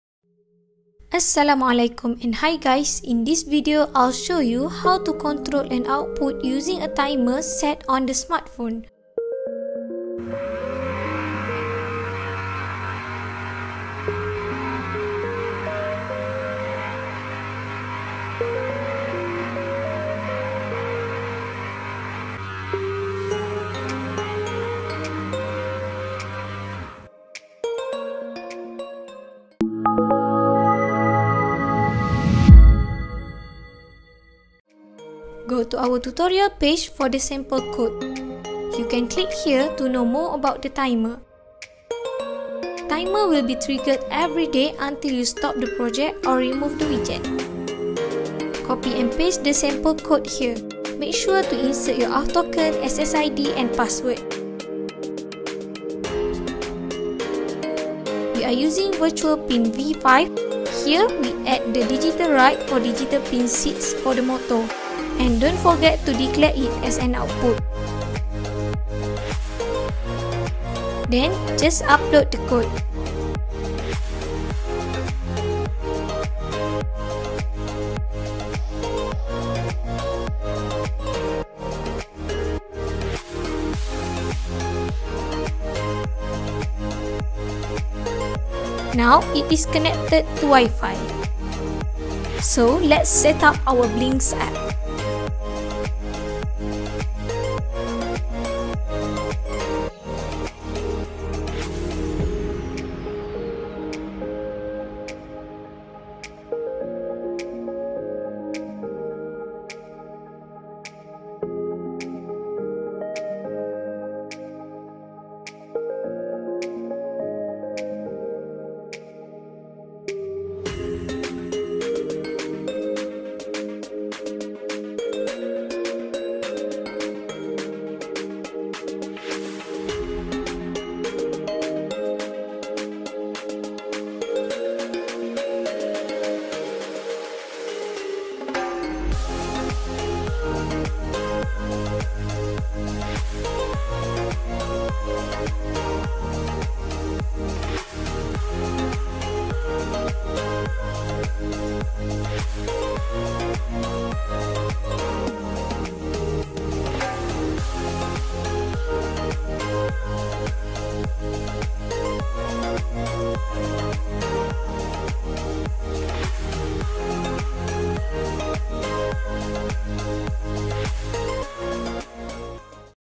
Control an Output with a Smartphone Timer: Lesson 4 Beginner IoT Project
In this lesson, we’ll show you how to control an output device using a timer set on your smartphone! ⏰📱 This simple project is designed especially for beginners interested in exploring the world of the Internet of Things (IoT). You’ll learn how to automate control of an output, making this a perfect starting point for your IoT journey.